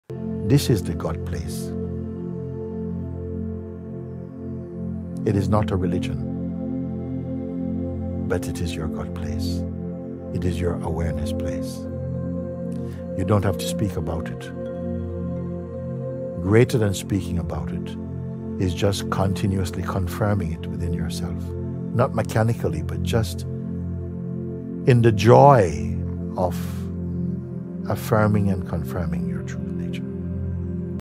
The Mind-Blowing Truth About Your God Place 😇🕊️🍃
Discover the profound wisdom of Mooji in this captivating short video. Immerse yourself in a collection of inspirational quotes from the renowned spiritual teacher, Mooji. Through beautifully curated visuals and thought-provoking words, this video takes you on a transformative journey of self-reflection and spiritual awakening. As you listen to Mooji's profound teachings, let his words resonate deep within your soul, inviting you to explore the essence of your true nature. Each quote unfolds like a gentle breeze, guiding you towards inner peace, self-discovery, and the realization of your infinite potential. Join us on this enlightening voyage and experience the power of Mooji's timeless wisdom.